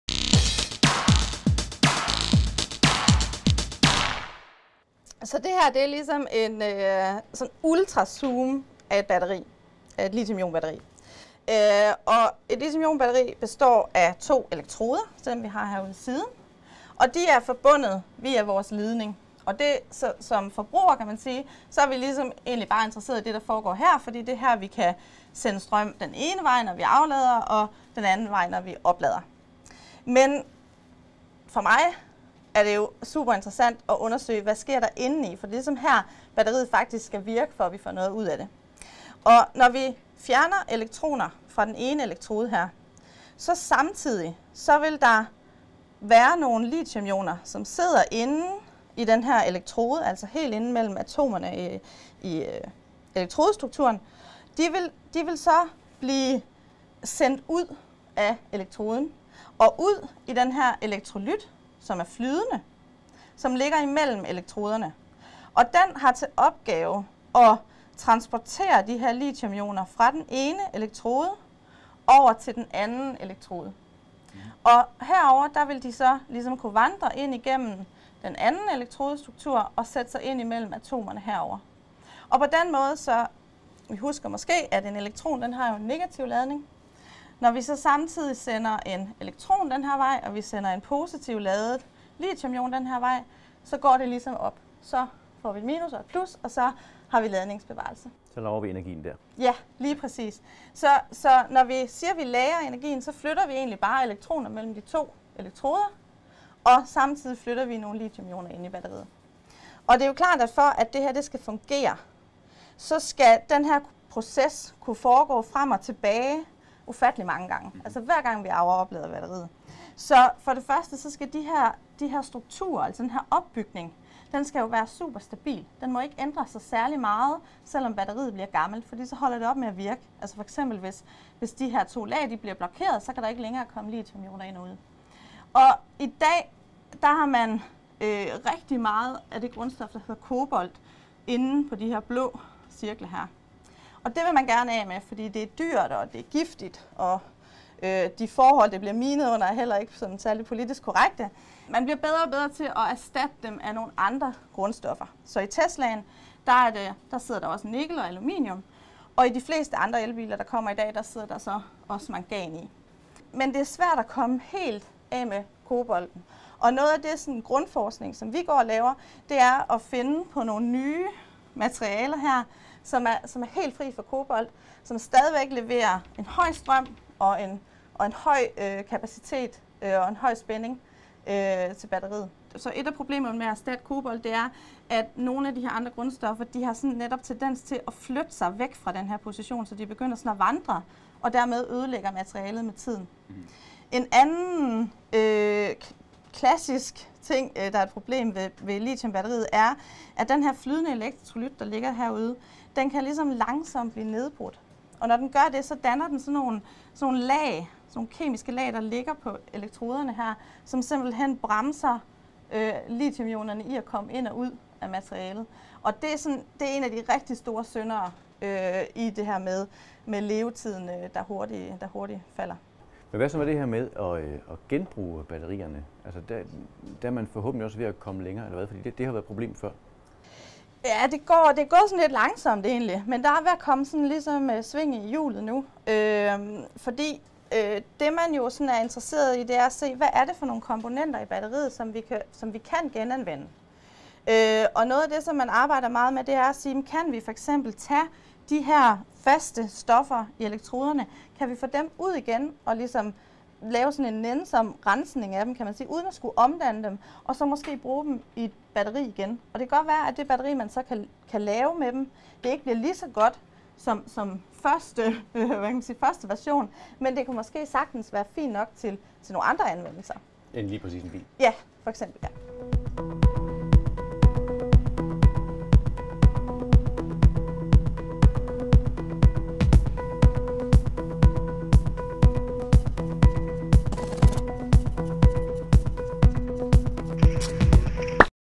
Hvordan virker lithiumbatterier?
Batteriforsker Dorthe Bomholdt Ravnsbæk forklarer hvordan batterier i elbiler virker og fortæller om udfordringerne ved at udvikle nye og bedre batterier.